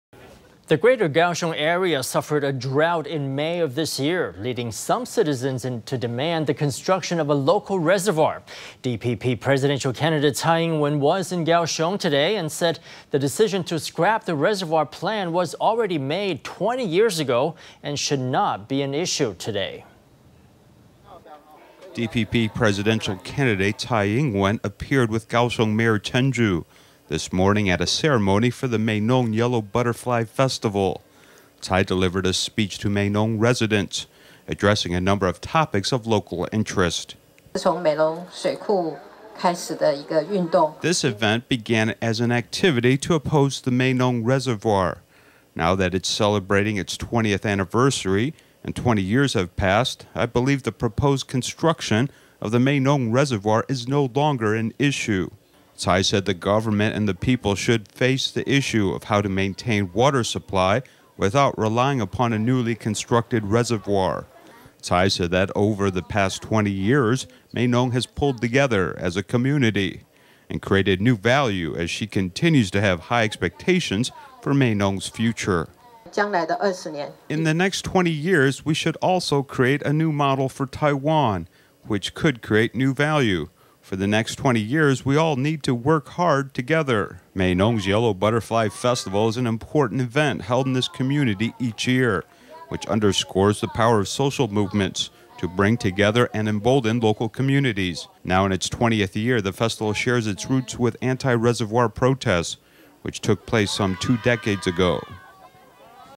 DPP presidential nominee Tsai Ing-wen visits Meinung Butterfly Festival to support local r...
DPP presidential nominee Tsai Ing-wen and Kaohsiung Mayor Chen Chu attended the Meinung Yellow Butterfly Festival this morning. The greater Kaohsiung area suffered a drought in May of this year, leading citizens to demand the construction of a local reservoir. Tsai said the decision to scrap the reservoir was already made 20 years ago and should no longer be an issue today. Later, Chen Chu called upon Meinung’s Hakka community to lend their support for Tsai who shares this community’s Hakka ancestry. DPP presidential candidate Tsai Ing-wen appeared with Kaohsiung Mayor Chen Chu this morning at a ceremony for the Meinung Yellow Butterfly Festival. Tsai delivered a speech to Meinung residents, addressing a number of topics of local interest.Tsai Ing-wenDPP Presidential NomineeThis event began as an activity to (oppose) the Meinung Reservoir. Now that it’s celebrating its 20th anniversary, and 20 years have passed, I believe the (proposed construction) of the Meinung Reservoir is no longer an issue.Tsai said the government and the people should face the issue of how to maintain water supply without relying upon a newly constructed reservoir. Tsai said that over the past 20 years, Meinung has pulled together as a community and created new value as she continues to have high expectations for Meinung’s future. Tsai Ing-wenDPP Presidential NomineeIn the next twenty years, we should also create a new model for Taiwan which could create new value.  For the next twenty years we all need to work hard together.Meinung’s Yellow Butterfly Festival is an important event held in this community each year which underscores the power of social movements to bring together and embolden local communities. Now in its 20th year, the festival shares its roots with anti-reservoir protests which took place some two decades ago.